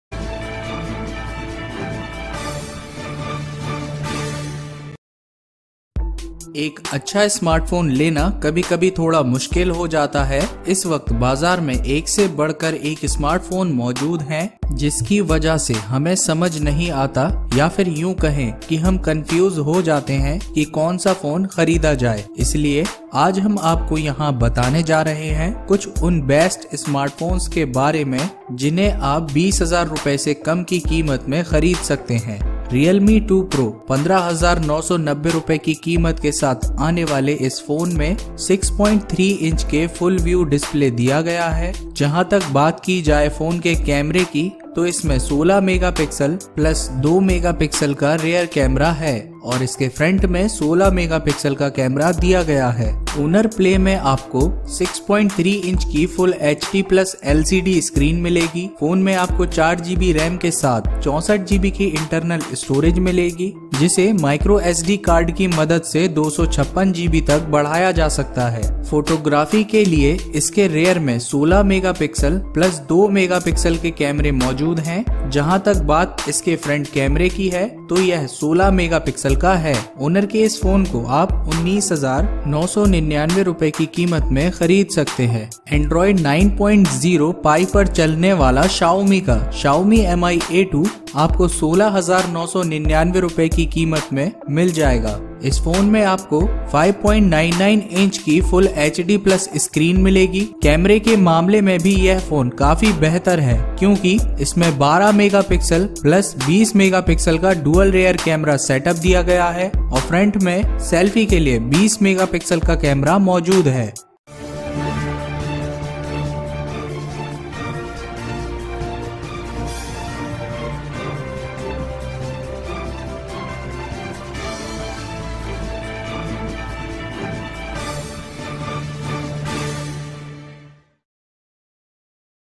खरीदना चाहते हैं बजट रेंज स्मार्टफोन तो ज़रूर देखें ये वीडियो | List of top budget rate smartphones
एक अच्छा स्मार्टफोन लेना कभी-कभी थोड़ा मुश्किल हो जाता है। इस वक्त बाजार में एक से बढ़कर एक स्मार्टफोन मौजूद हैं जिसकी वजह से हमें समझ नहीं आता कि कौन सा फोन खरीदा जाए। आज हम कुछ उन बेस्ट स्मार्टफोन्स के बारे बताएंगे में जिन्हें आप 20,000 रुपये से कम की कीमत में खरीद सकते हैं।